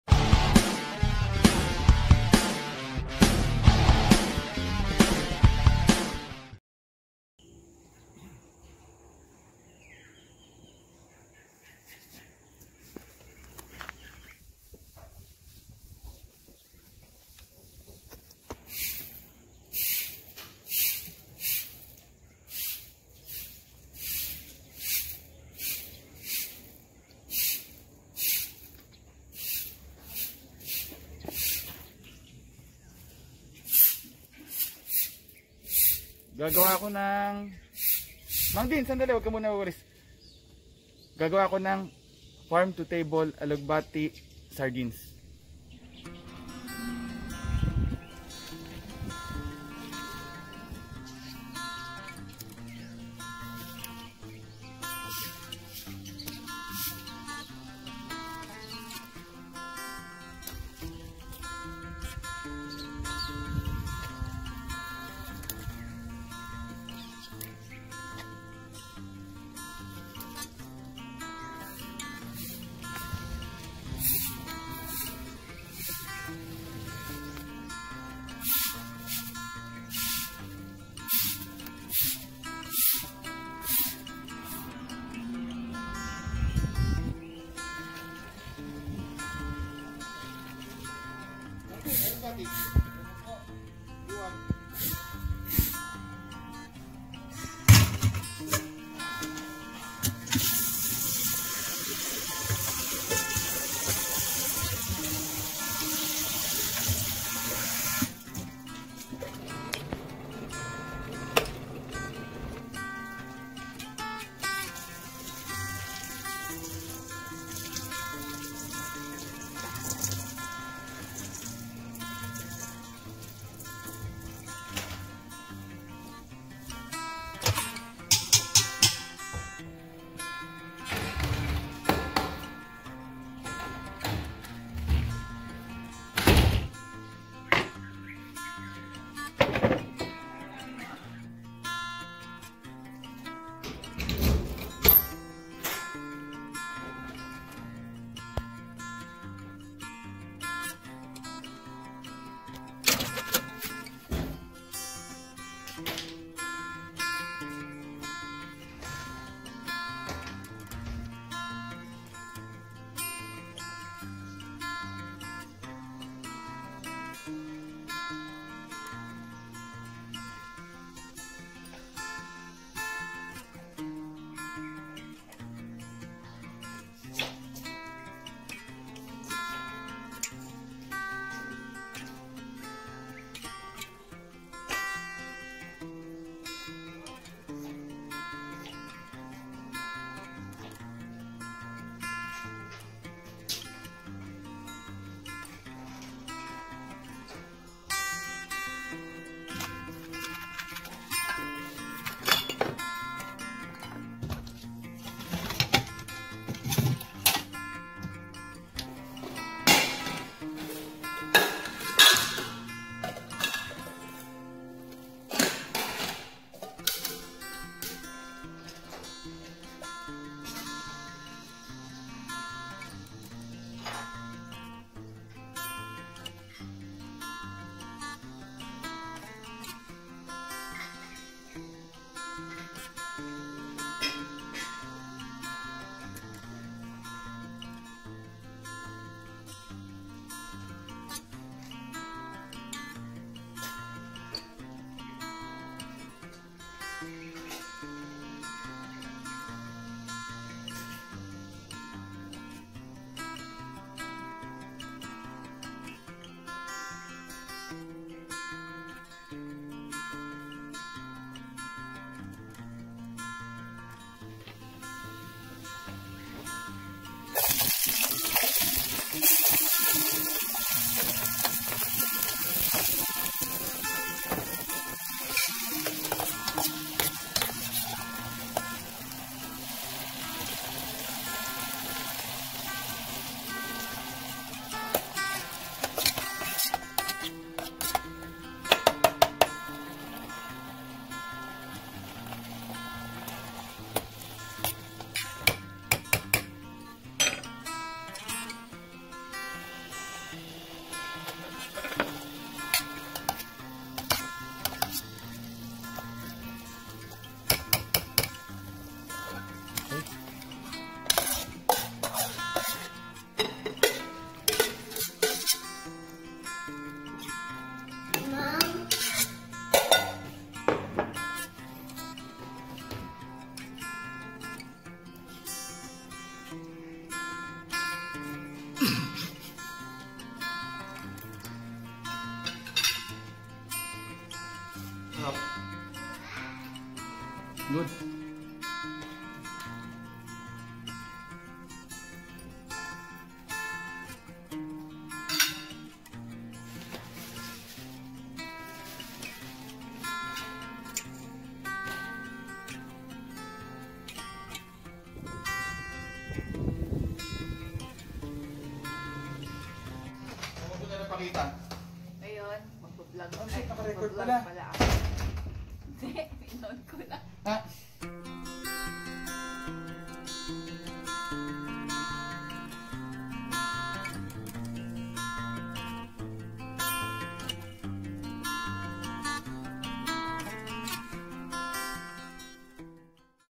Farm to Table Alugbati | Cooking Sardines with Fresh Alugbati Using My Homemade Alcohol Stove
I got some fresh Alugbati, also known as Malabar Spinach or Basella, from Neri's Garden, and cooked it with sardines, using my camping pots and my homemade alcohol stove.

Farm to table. Backyard Farming. Vegetable Garden.